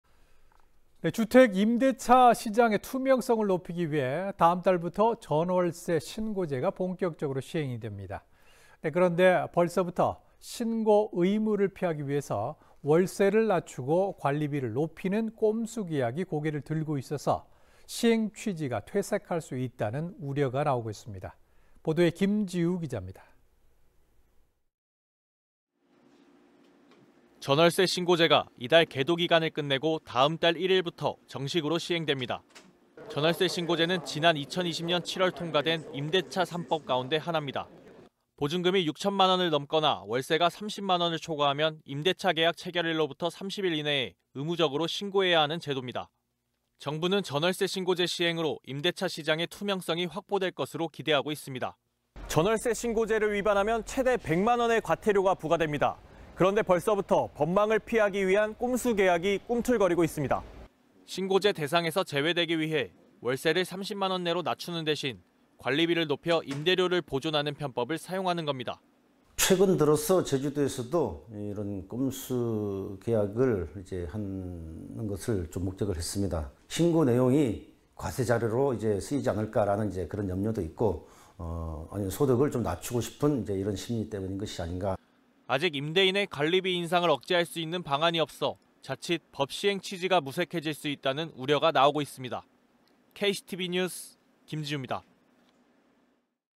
전월세 신고제 내달 시행…꼼수계약 '꿈틀'
주택 임대차 시장의 투명성을 높이기 위해 다음달부터 전월세 신고제가 본격적으로 시행됩니다.

그런데 벌써부터 신고 의무를 피하기 위해 월세를 낮추고 관리비를 높이는 방식의 꼼수계약이 꿈틀거리면서 법 취지가 무색해질 수 있다는 우려가 나오고 있습니다.